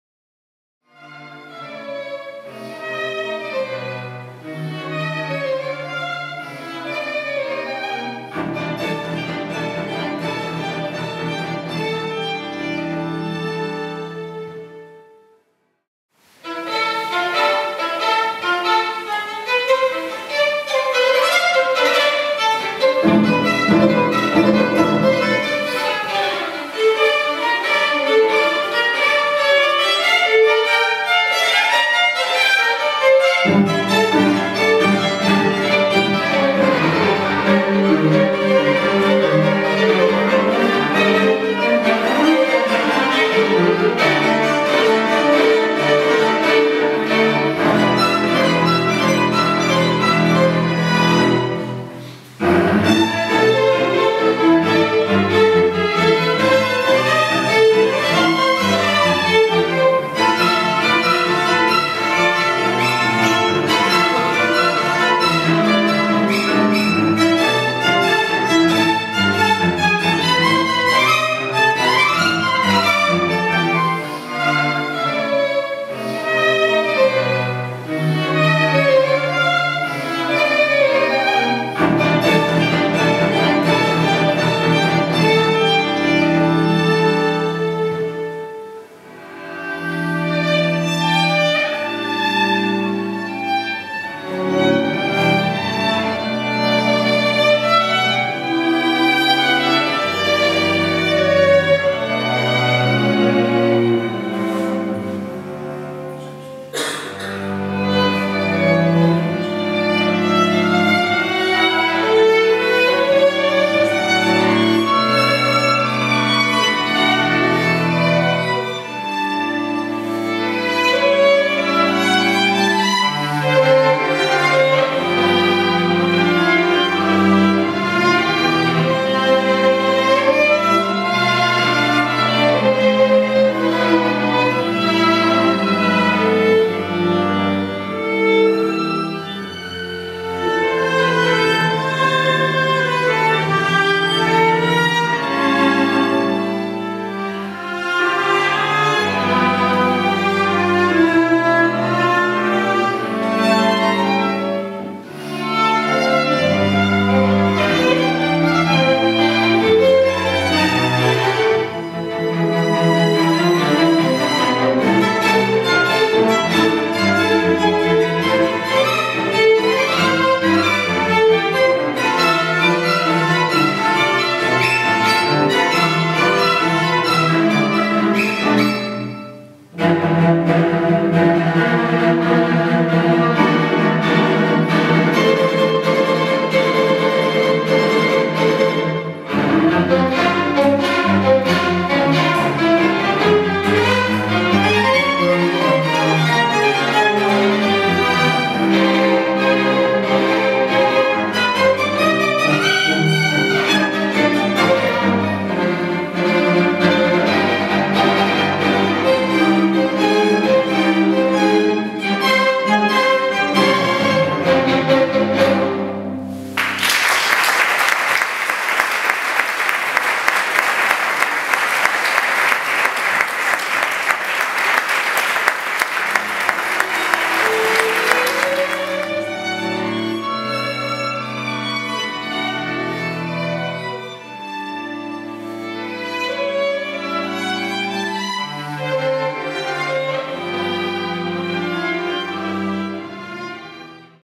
CUARTETO ARTIS ITER - SONIDOS DE INVIERNO - MÚSICA EN NAVIDAD V - 29.12.12
Bis con música de Piazzola del Concierto del Cuarteto Artis Iter en la iglesia de Barrillos de Curueño dentro del ciclo Sonidos de Invierno - Música en Navidad V, que organiza la Fundación Cerezales, Antonino y Cinia, y que tuvo lugar el sábado 29 de diciembre´12.